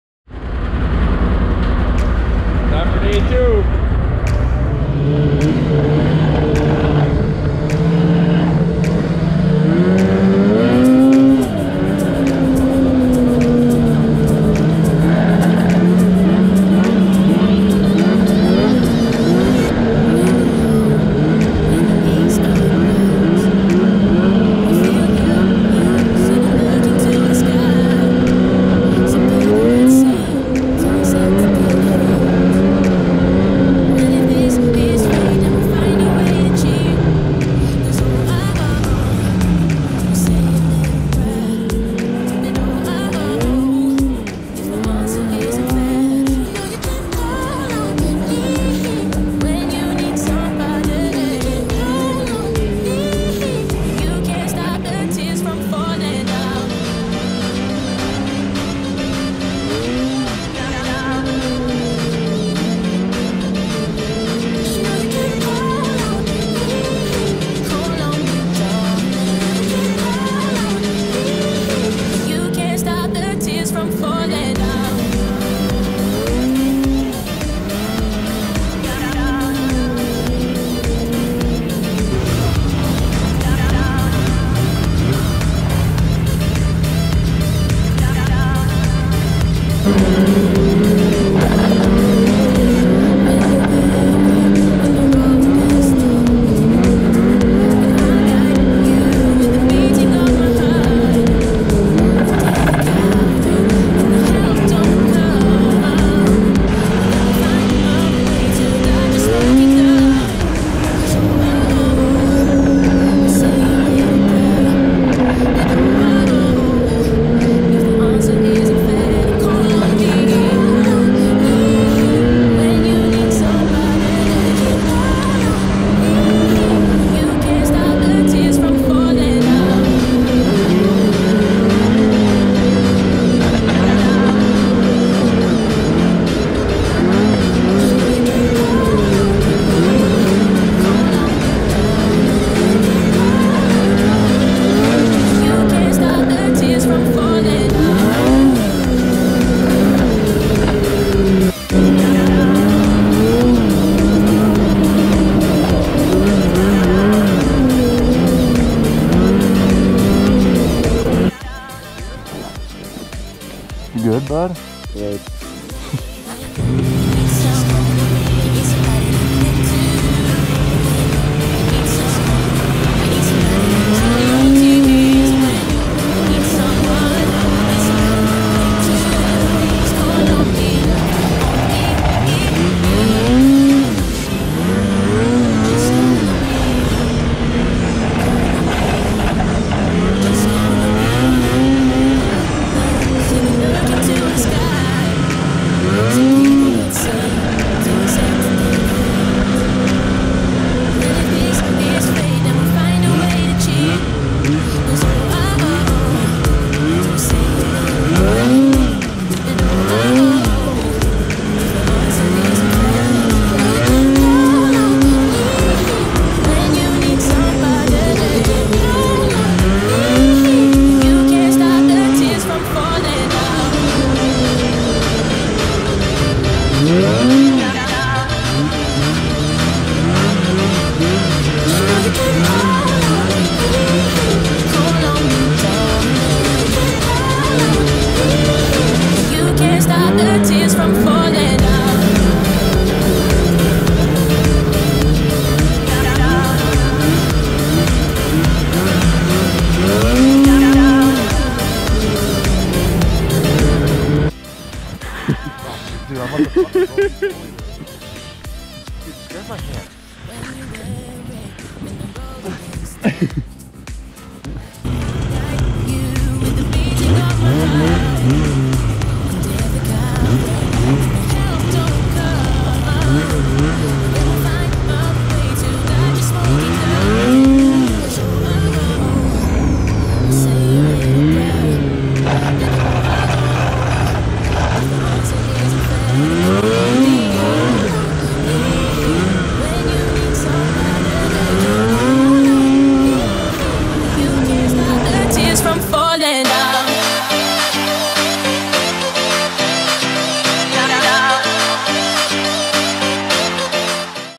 Snowmobiling Northern Wisconsin | Last day!
Another lack of footage due to low temperature conditions! Still wanted to get a video out, lots of trail riding, little messing around.

Hope to get up north one more time.